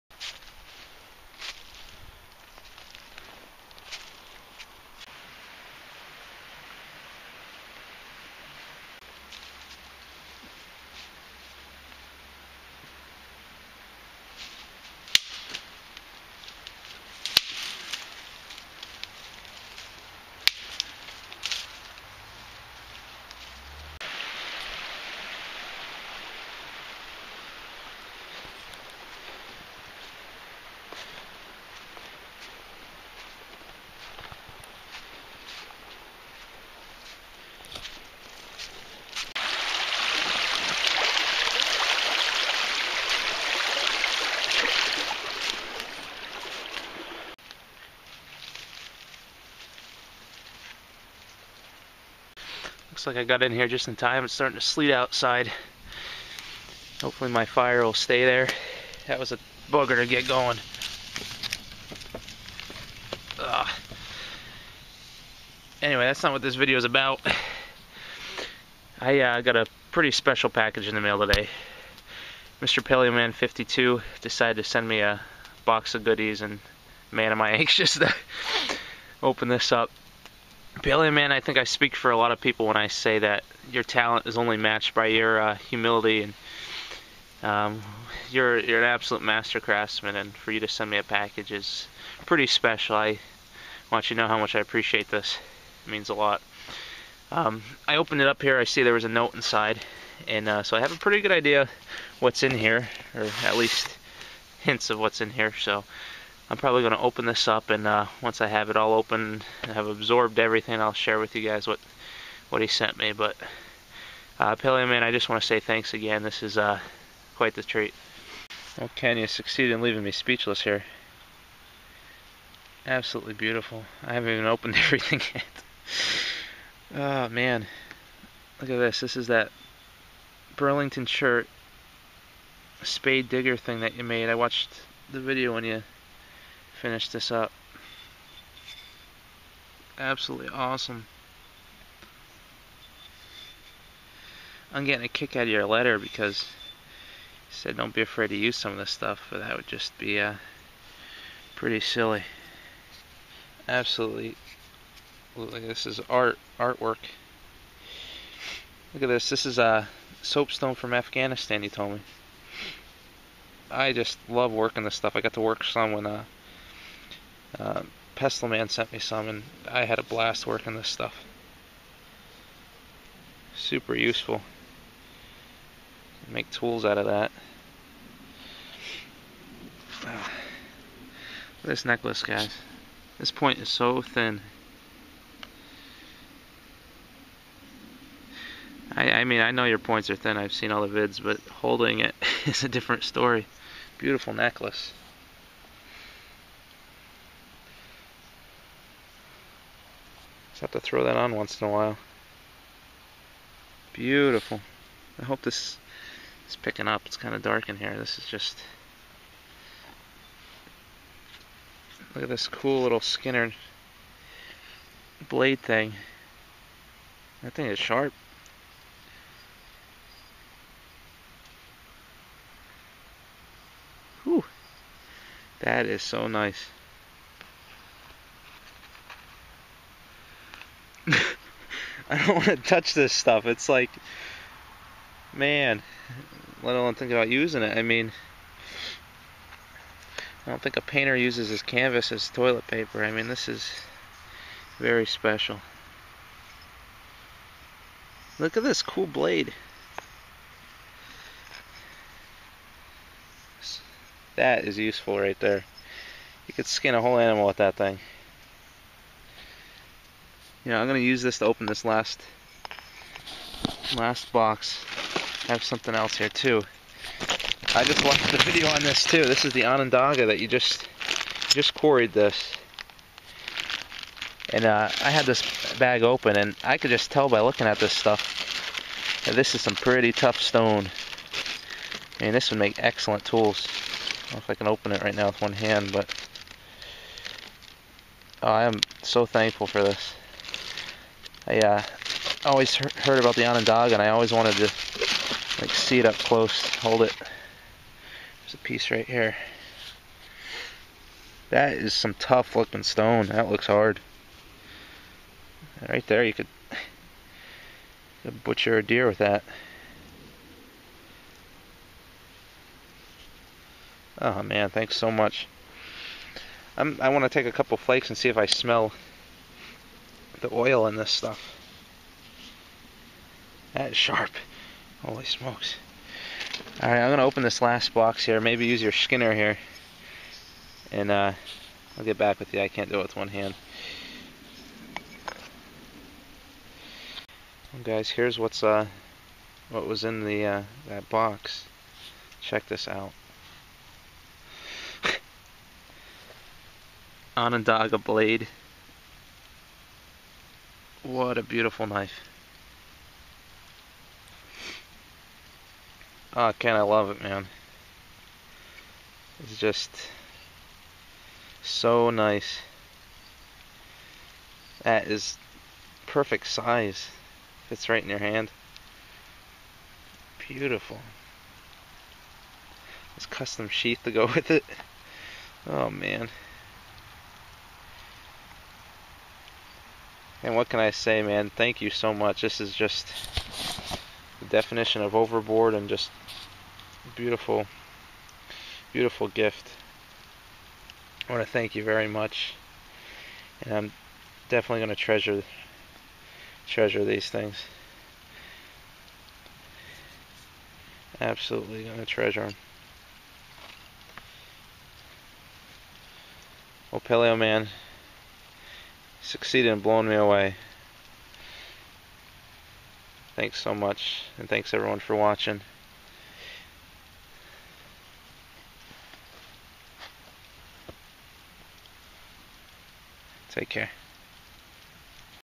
TheTribeOfBenjamin Thanks Paleoman52 !
Wanted to share with everybody this incredible gift package that Paleoman52 sent me! So special! Thank you Ken! I wish everyone a happy Thanksgiving!